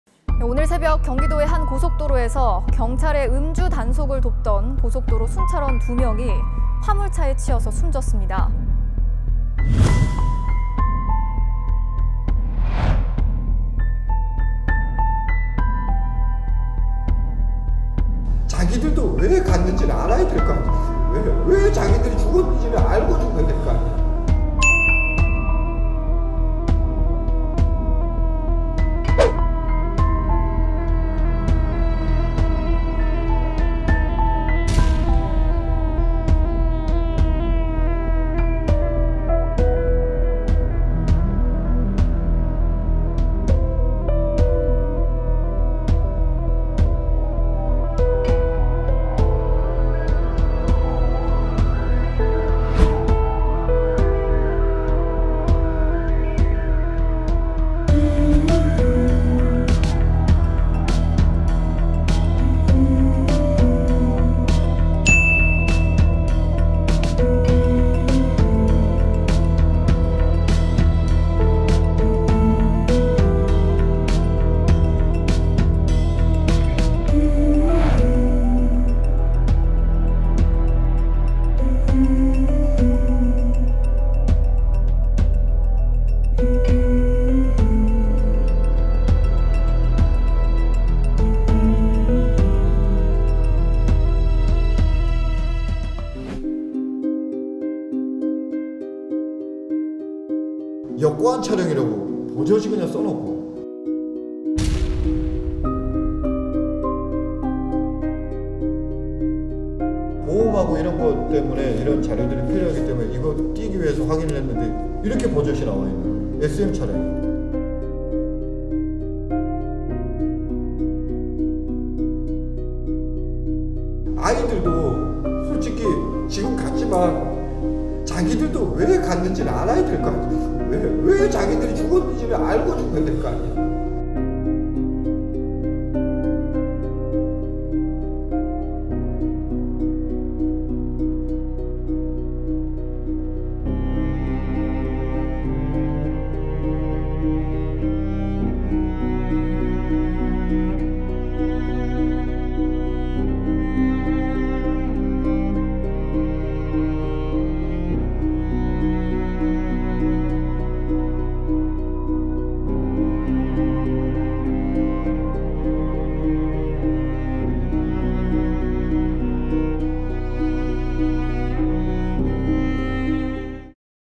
'트레일러에 치인 순찰원 또 다시 치고 지나간 승용차' 사인 미궁에 빠져
얼마 전 고속도로에서 일하다 숨진 20대 순찰대원 2명의 사고 소식 보도해드렸는데요. MBC 취재 결과, 당시 사고를 당한 순찰대원 한 명이 트레일러에 치인 뒤 다른 승용차에 또 한 번 받힌 사실이 새롭게 드러났습니다.